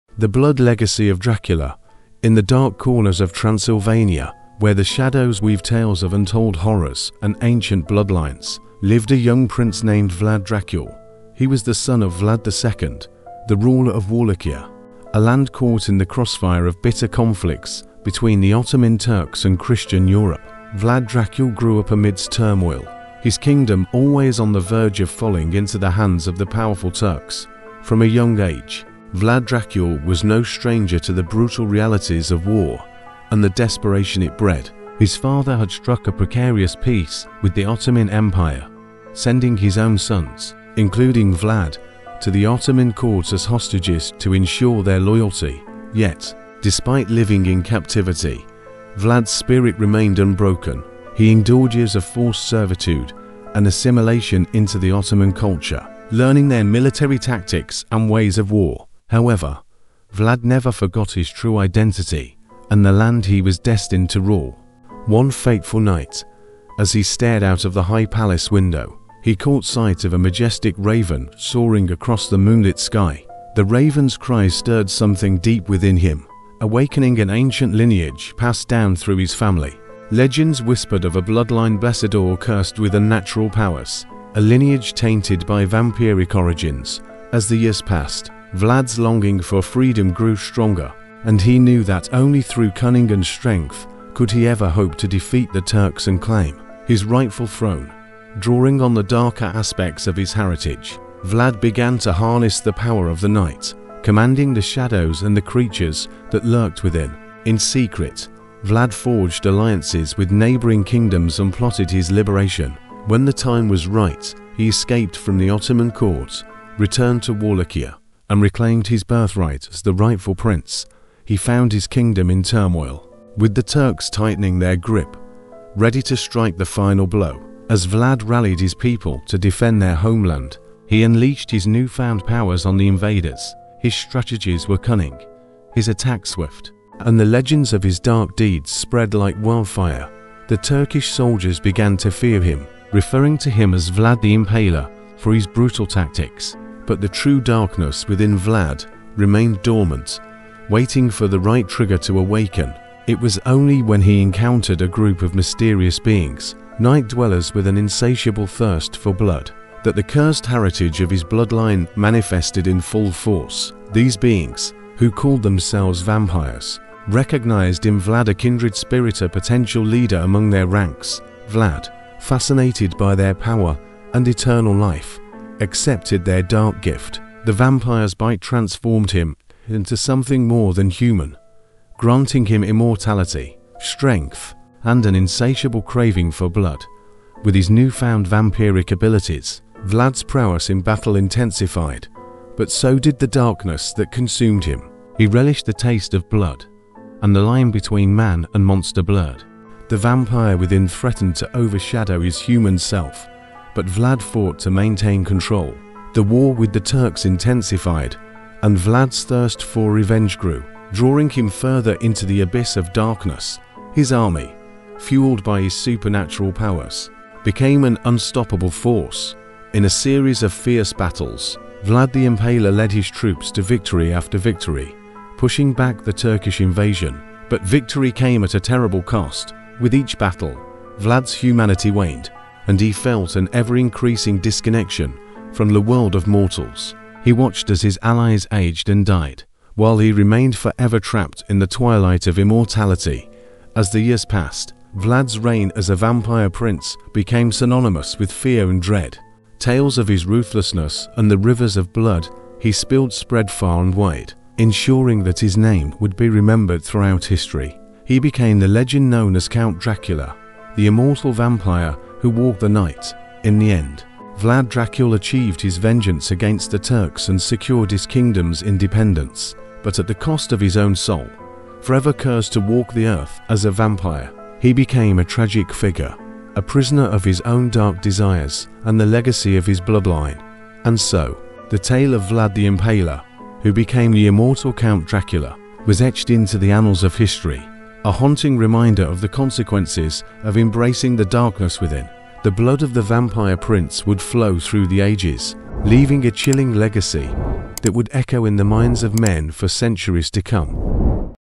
The Blood Legacy of Dracula : what if he really was a vampire?
Prepare to venture into the realm of dark fantasy as "The Blood Legacy of Dracula" takes you on a mesmerizing journey through history, folklore, and the immortal world of vampires. This captivating fantasy documentary unravels the enigmatic tale of the legendary Count Dracula, exploring the enduring legacy of the vampire who has both terrified and fascinated generations.

In this riveting exploration, we delve into the origins of the Dracula myth, tracing its roots back to the historical figure Vlad III, also known as Vlad the Impaler. Through expert interviews with historians, folklore experts, and literary scholars, we uncover the fascinating confluence of fact and fiction that shaped the iconic vampire figure.

As the documentary delves into the world of fantasy, we encounter the legend of Dracula, an immortal being cursed with a thirst for human blood. Through stunning visualizations and evocative reenactments, we bring to life the haunting and ethereal ambiance of Transylvania, where the ominous Castle Dracula stands as a sentinel of time.

But "The Blood Legacy of Dracula" doesn't stop at the surface of the vampire myth; it delves into the psychological aspects that have drawn people to this dark and alluring creature. Scholars and psychologists weigh in on the psychological fascination with vampires, discussing how Dracula embodies our fears, desires, and even our longing for immortality.

Throughout the documentary, we are guided by the engaging narration of a seasoned storyteller, weaving together historical facts, fantastical tales, and contemporary insights. The visual journey is enriched with stunning animations that capture the essence of the supernatural, unveiling the hidden truths behind the vampire myth.

As the narrative reaches its apex, "The Blood Legacy of Dracula" reveals the enduring impact of this timeless vampire story on literature, cinema, and popular culture. From Bram Stoker's classic novel "Dracula" to the countless adaptations in film and television, we witness the evolution of the vampire trope and its continued resonance in modern storytelling.